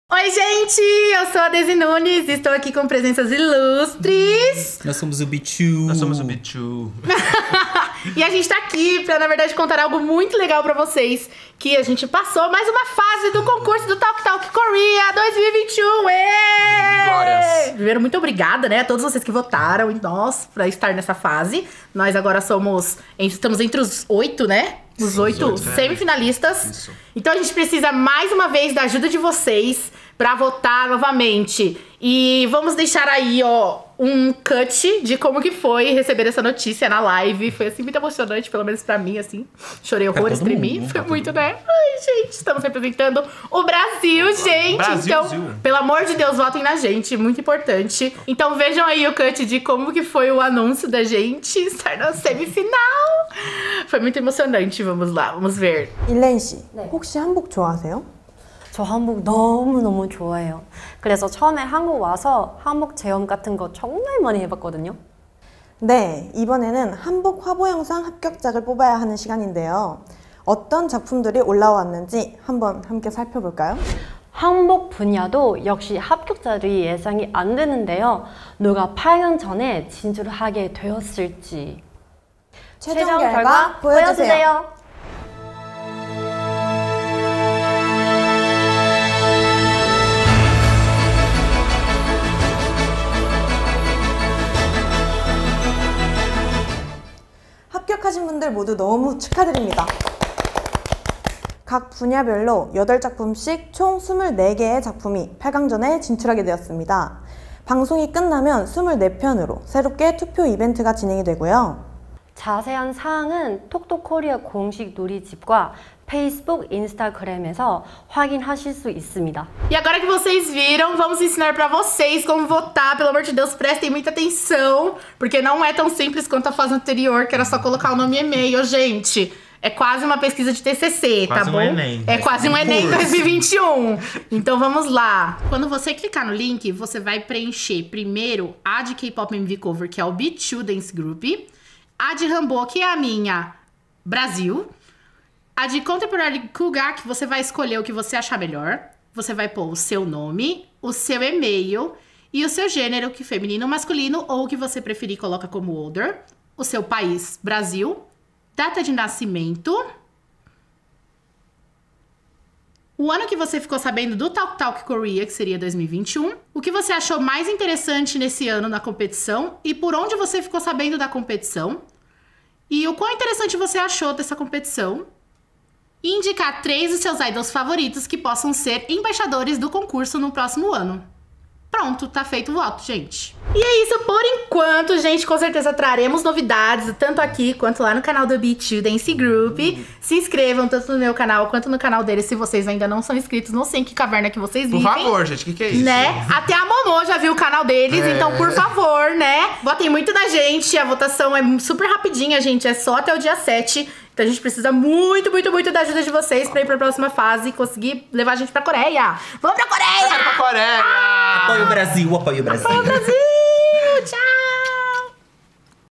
ME LEVEM PRA COREIA COM O B2! TOP8 talktalkkorea2021 SEMIFINALISTAS BRASILEIROS!
ME LEVEM PRA COREIA COM O B2! talktalkkorea2021 SEMIFINALISTAS BRASILEIROS!

PASSAMOS PRA ETAPA TOP 8 DO CONCURSO QUE PODE LEVAR A GENTE PRA COREIA! PRA AJUDAR A REALIZAR NOSSO SONHO, VOTEM EM MIM E NO B2 DANCE GROUP!

A VOTAÇÃO É SÓ ATÉ DIA 7 PORTANTO.

VOTEEEEEEMMM! 💥🗣

B2 Dance Group: @b2dancegroup